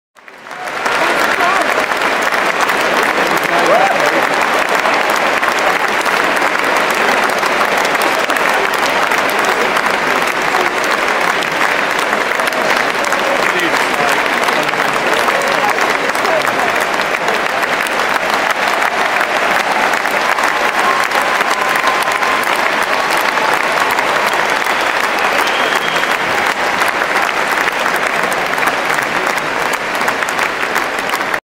Reilly in the Royal Box with Prince Charles
After winning the Juniors singles title at Wimbledon on Sunday, my very talented nephew, Reilly Opelka was presented with the trophy by members of the Royal Family.